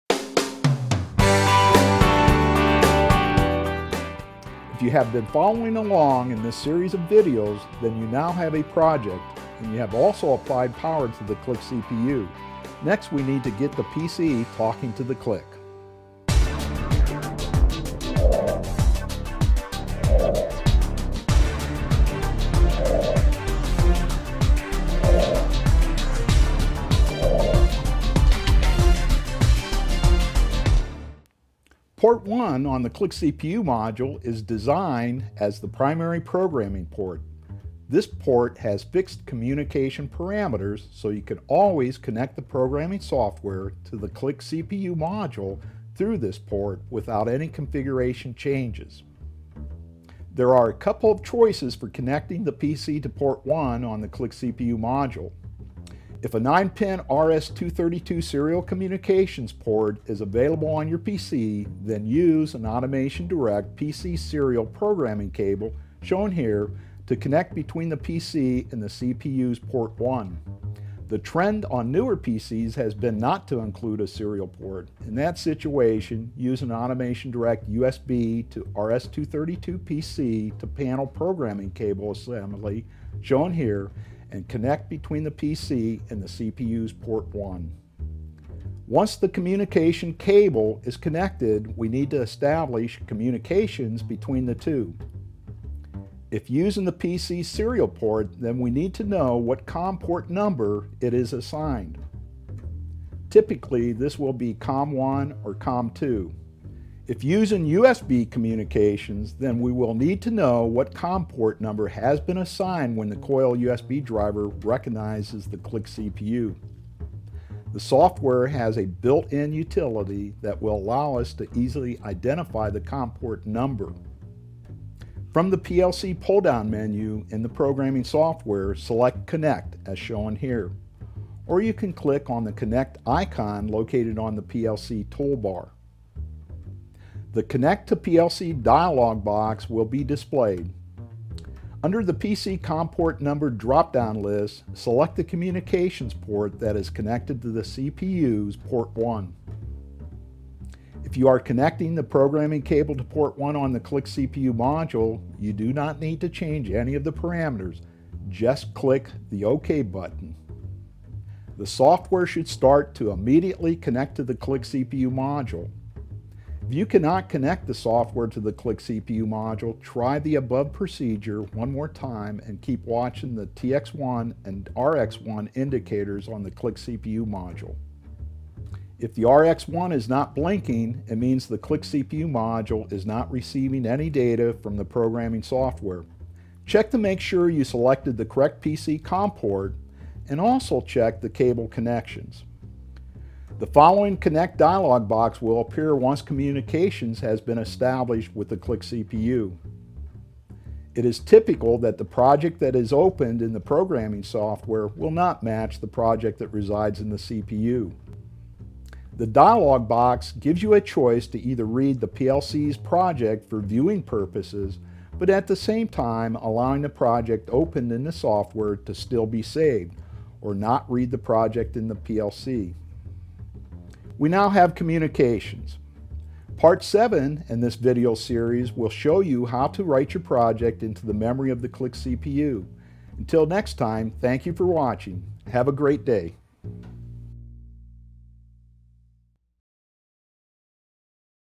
CLICK PLC - How to Establish PC to PLC Communications (Part 6) from AutomationDirect
(VID-CL-0011)- Video Tutorial - CLICK PLCs - Getting Started - Part 6, Establish PC to PLC Communications